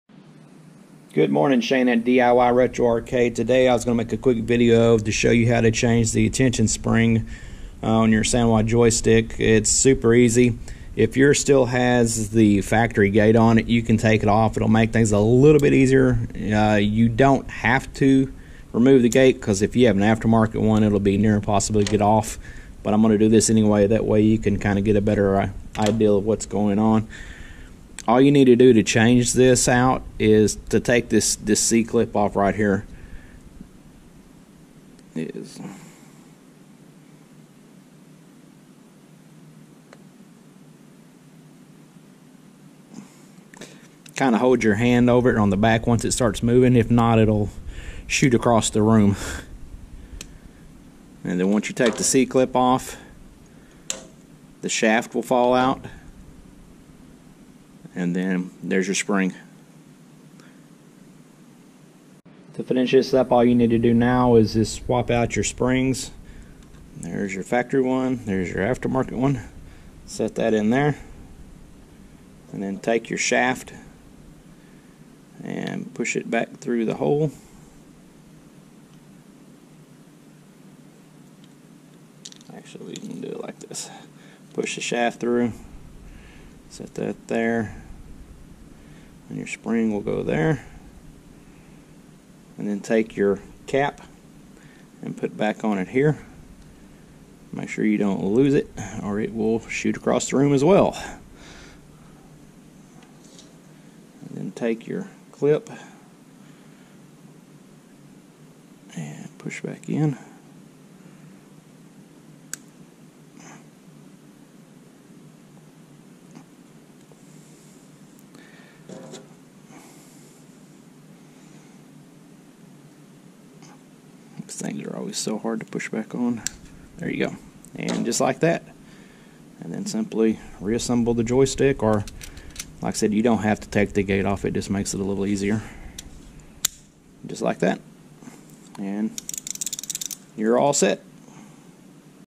How To Change Your Sanwa JLF Joystick Tension Spring In About 2 Minutes
Changing out the tension spring in a sanwa joystick is super easy. All you need is a small flat blade screw driver and about 2 minutes.